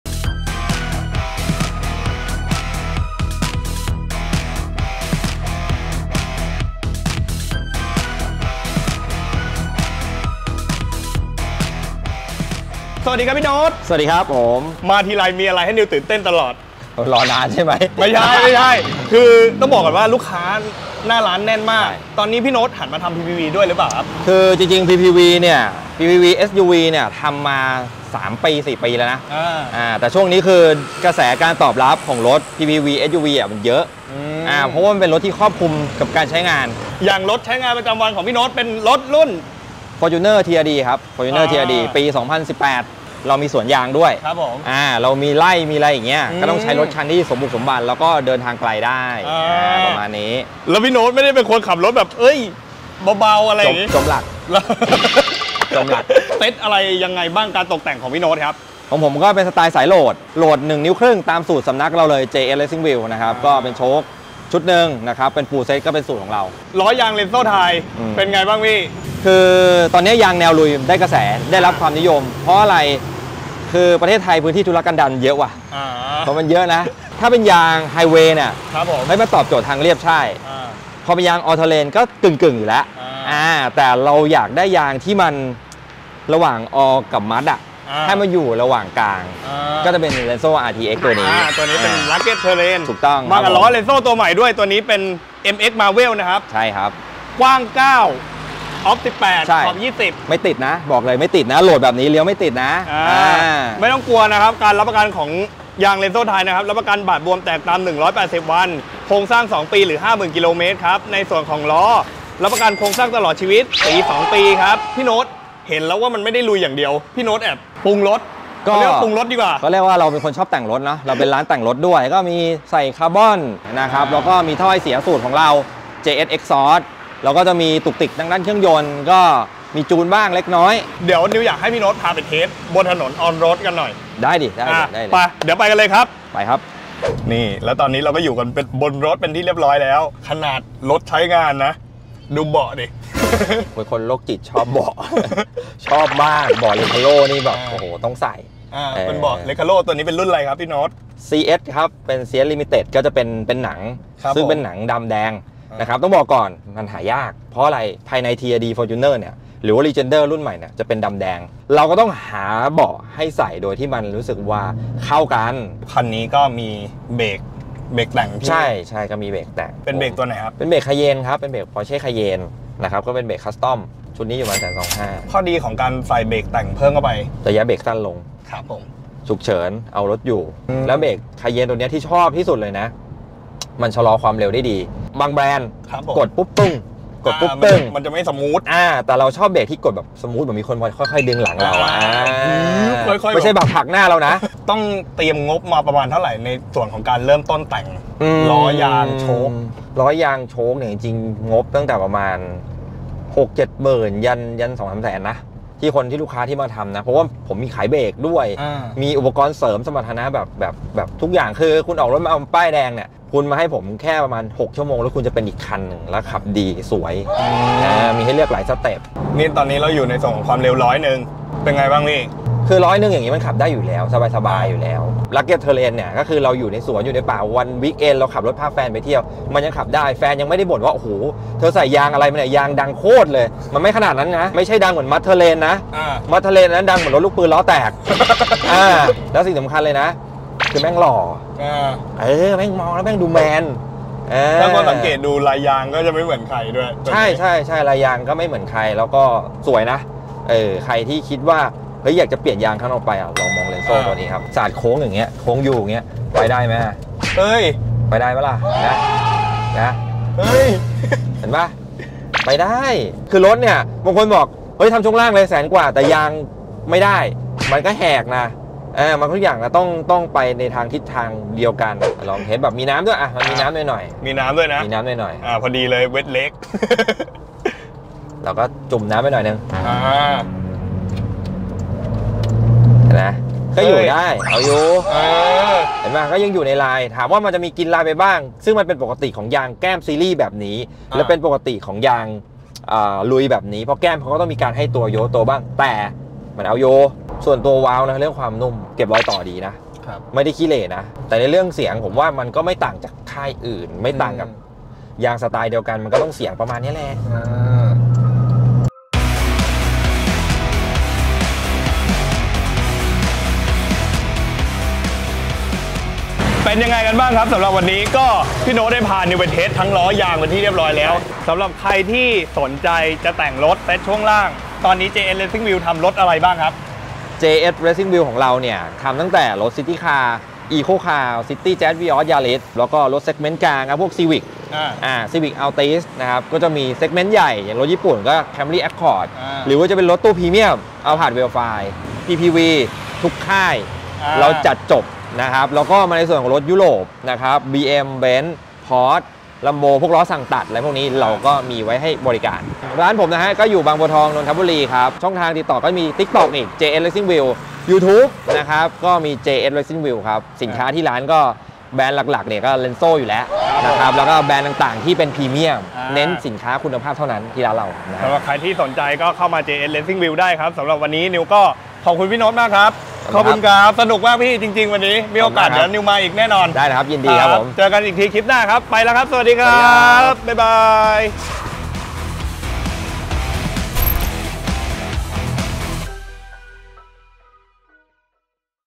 พาไปเจาะรถพี่โน๊ต เจ.เอส เรซซิ่งวีล ปรุงรถออกมาได้กลมกล่อมแค่ไหน ตามไปชม!!
จะเป็นอย่างไร เมื่อ พี่โน๊ต จาก เจ.เอส เรซซิ่งวีล มากระทบไหล่ นิว เลนโซ่
พาเจาะสเต็ปรถแต่งคู่ใจของพี่โน๊ตจากสำนัก เจ.เอส งานนี้บอกหมดแบบไม่มีกั๊ก
จะดุเด็ดเผ็ดมันส์แค่ไหน ตามไปชมกันเลยครับ

ดูล้อเพิ่มเติมได้ที่
ดูยางเพิ่มเติมได้ที่
ดูคลิปใหม่ๆได้ที่ช่อง Lenso Channel